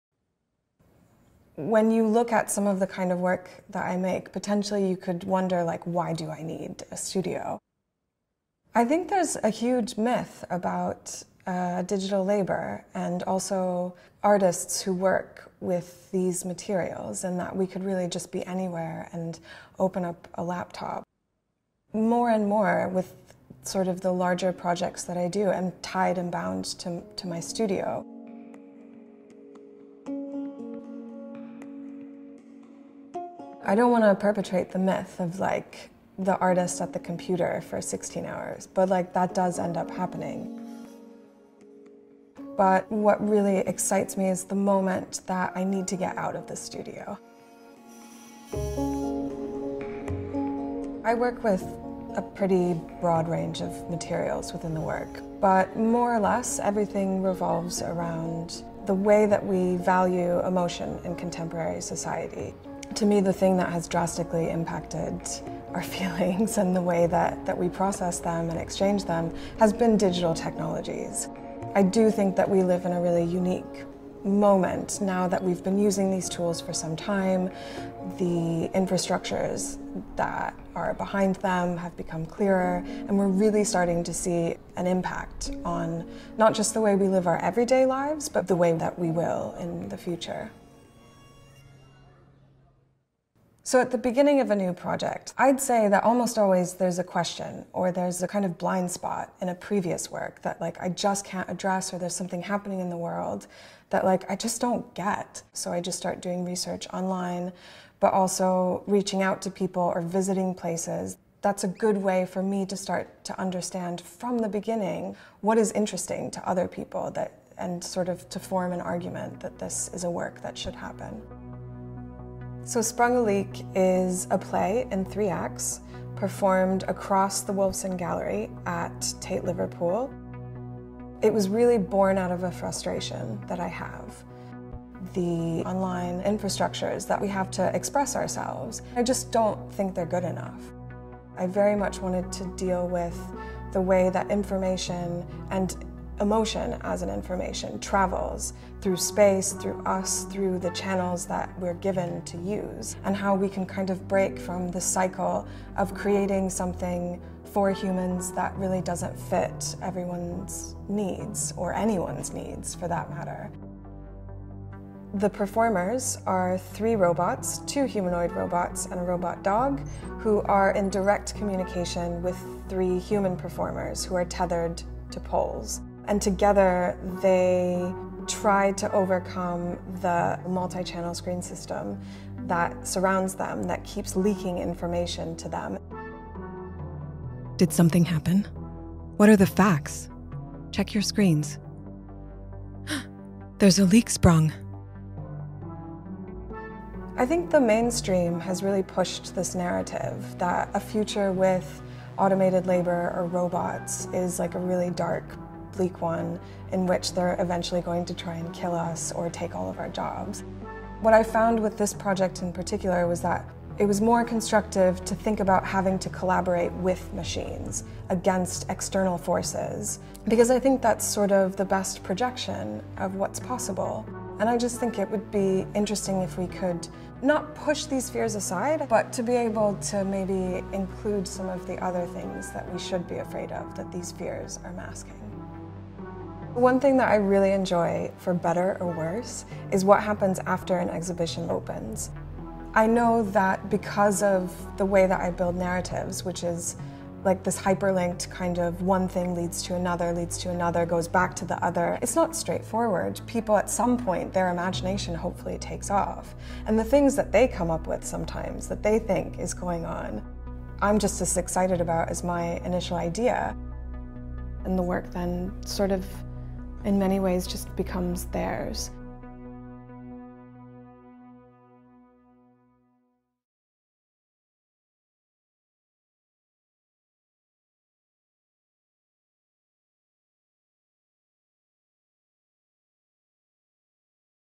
Cécile B. Evans – 'We Live in a Really Unique Moment Now' | TateShots
Cécile B. Evans’s Tate Liverpool commission 'Sprung a Leak' is a multi-dimensional work featuring two humanoid robots and a robot dog performing in the gallery.

'Sprung a Leak' 2016 explores the movement of data, artificial intelligence, and the relationship between humans and machines.

The installation at Tate Liverpool examines the increasing influence that new technologies have on how we feel and act.